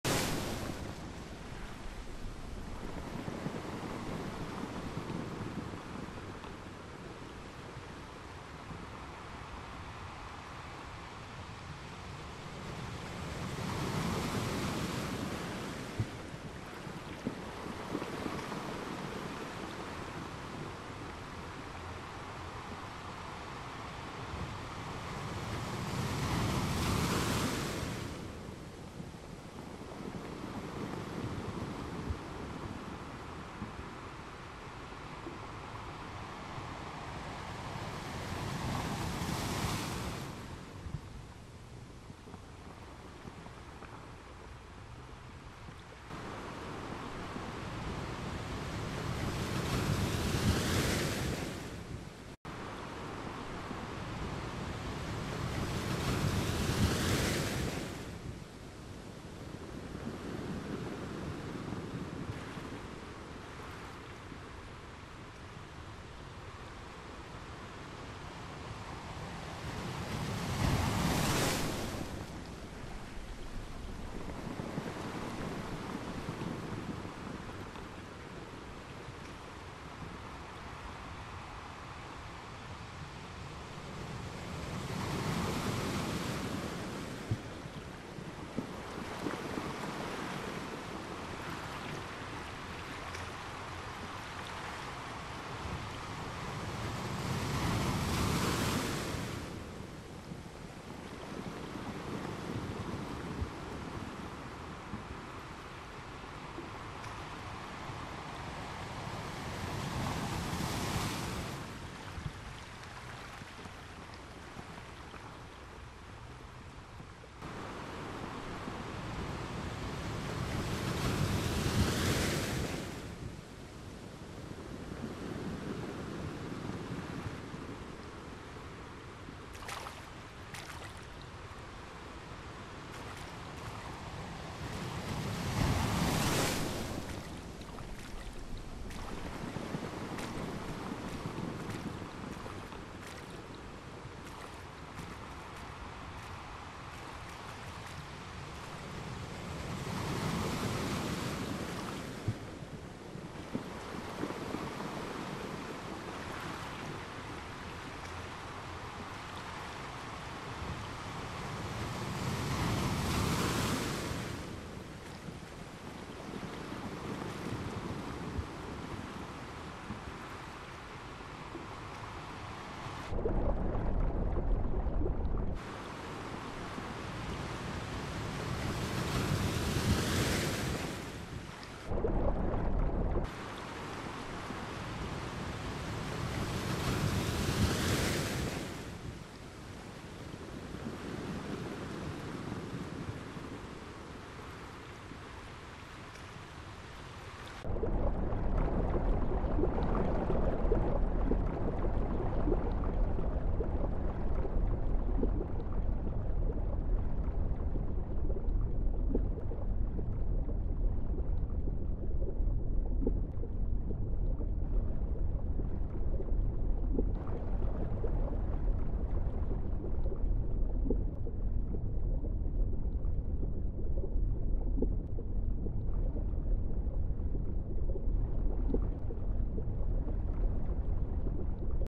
Fluid Flux 3.0 - Underwater tunnel [Unreal Engine 5]
It’s been over a year since the last major update to Fluid Flux, and while I often call updates ‘big,’ this one truly stands out. The update 3.0 is massive, bringing significant improvements in performance, stability, and memory management, along with many new features and enhancements.